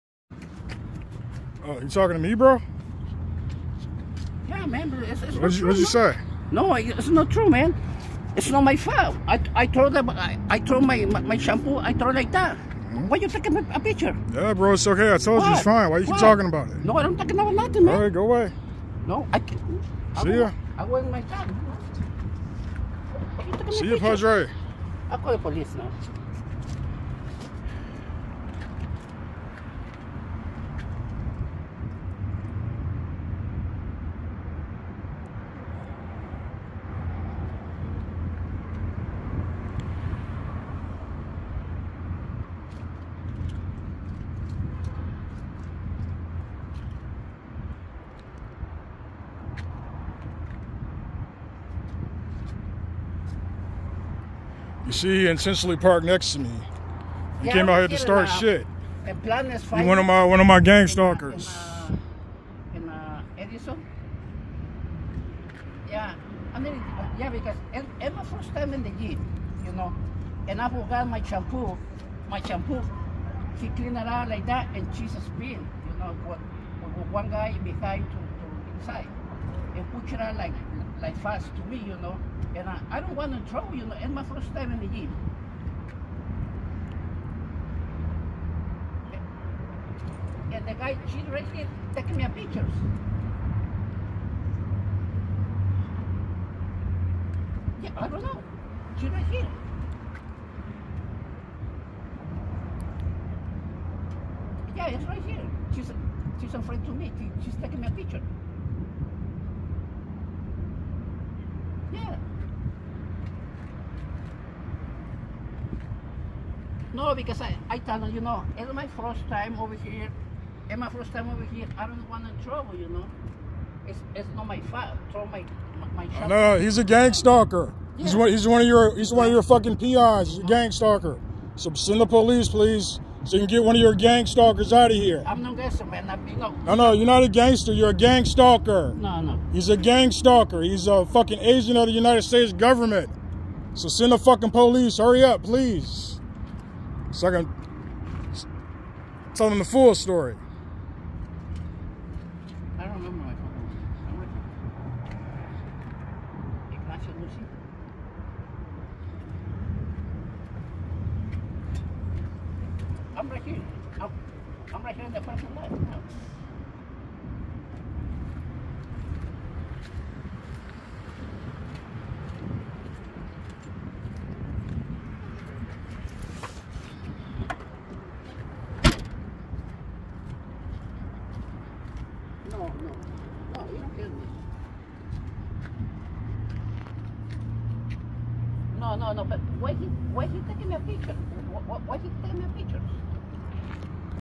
the Gangstalking program sent one of their peons to stalk me at the Planet Fitness in Addison, IL.
So, this peon tried to start a fight with me in the locker room at Planet Fitness (Addison, IL). NOTICE: he was already parked backwards (which is one of their hallmarks) next to me.